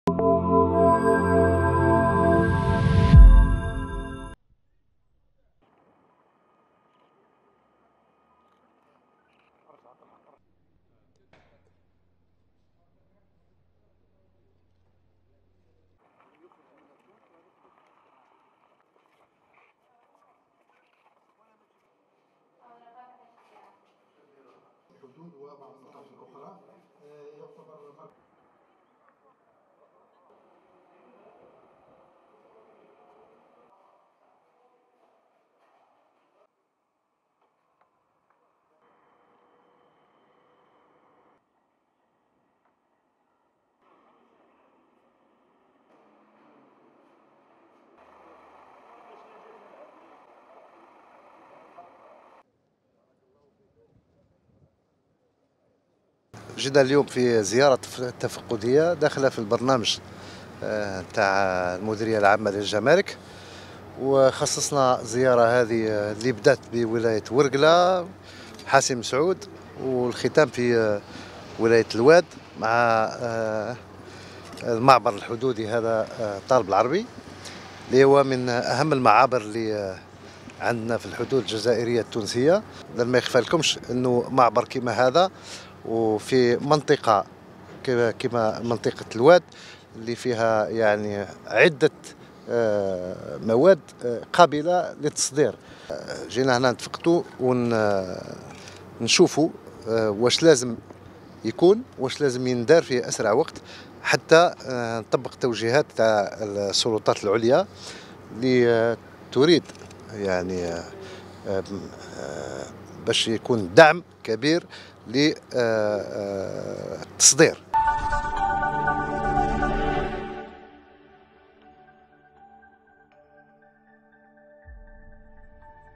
وادي سوف: المدير العام للجمارك في زيارة تفقدية لمرافق قطاعية بالولاية
وادي سوف(وأج)- في إطار جولة تفقدية، قادته إلى عدة مناطق بالجنوب الشرقي، شرع المدير العام للجمارك الجزائرية اللواء عبد الحفيظ بخوش، في زيارة رسمية إلى ولاية وادي سوف، أين توقف على سير العمل الجمركي في المعبر الحدودي الطالب العربي، كما استمع  اللواء بخوش لمقترحات العاملين بالمركز وانشغالاتهم .